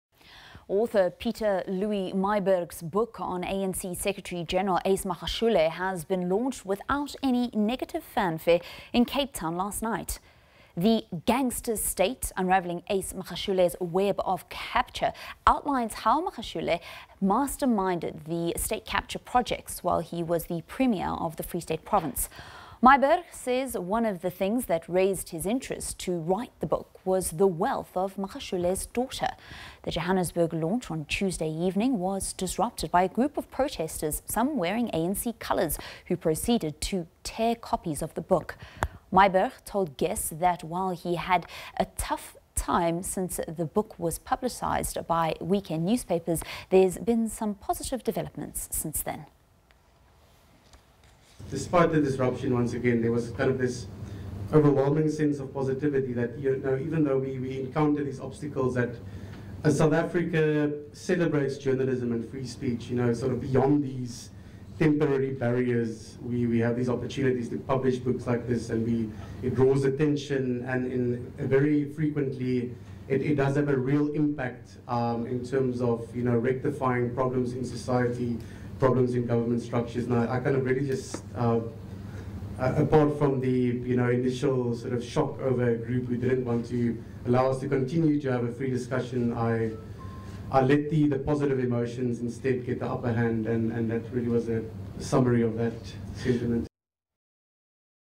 Gangster state book launched without negativity in Cape Town
Author Pieter-Louis Myburgh's book on ANC Secretary General Ace Magashule has been launched without any negative fanfare in Cape Town Thursday night.